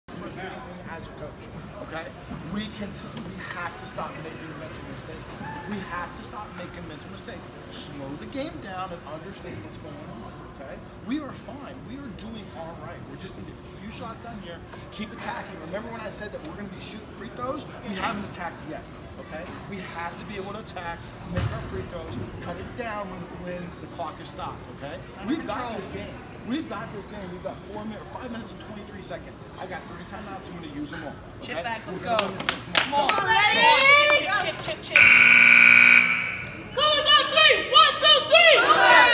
Westland’s Matt Jaynes
Westland girls basketball coach Matt Jaynes talks to his team during a 61-56 loss at Walnut Ridge on Dec. 13. [Frank DiRenna/ThisWeek]